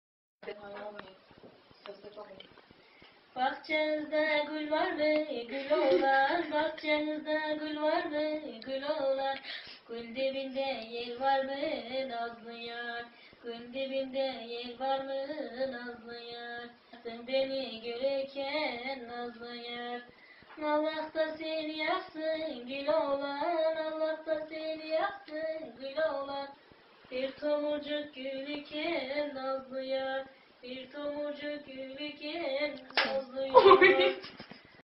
Oğlunu Tokatlayan Anne
Şarkı söylerken oğlu tarafından sabotaja maruz kalan anne bakın nasıl tokaat atıyor...